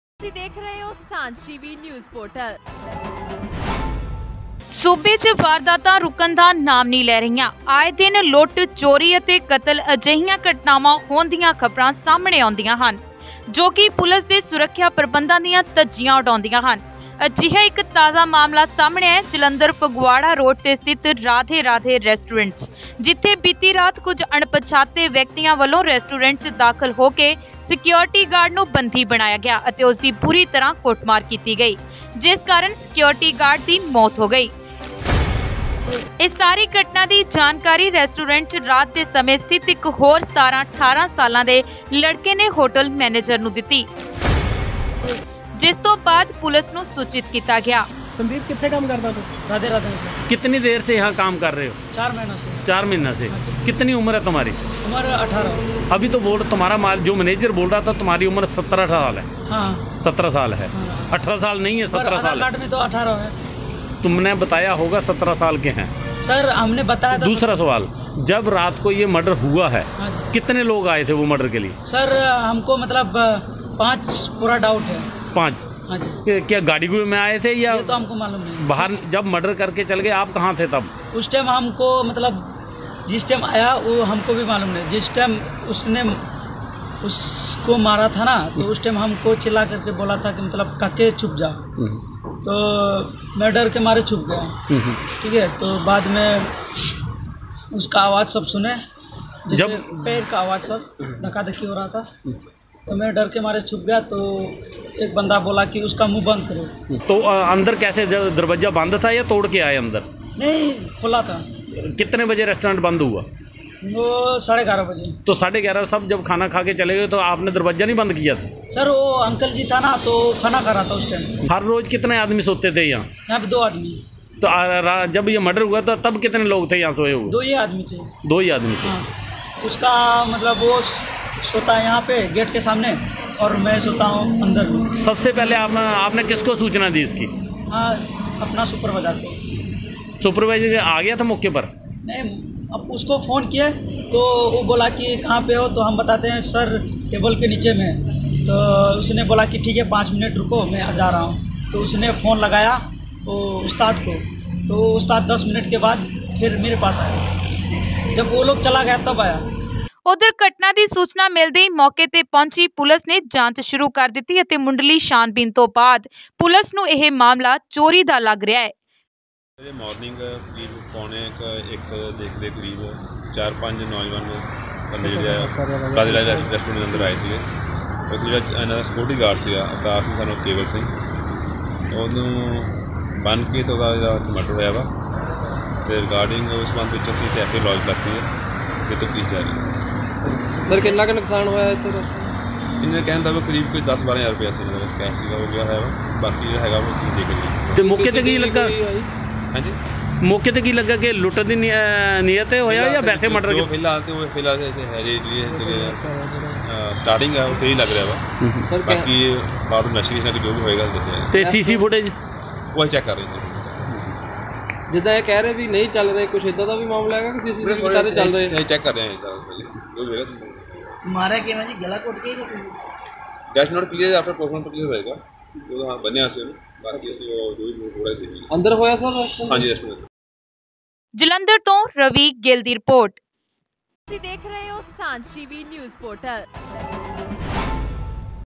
ਜਲੰਧਰ ਫਗਵਾੜਾ ਰੋਡ 'RADHE RADHE RESTAURENT ਵਿਚ SECURITY GAURD ਦਾ ਹੋਇਆ ਕਤਲ ,POLICE ਨੇ ਕੀਤੀ ਜਾਂਚ ਸ਼ੁਰੂ
ਜਲੰਧਰ ਫਗਵਾੜਾ ਰੋਡ 'ਤੇ ਵੱਡੀ ਵਾਰਦਾਤ, ਹੋਟਲ ਦੇ ਸਿਕਿਓਰਟੀ ਗਾਰਡ ਦਾ ਹੋਇਆ ਕਤਲ

Marriage Ceremony!!!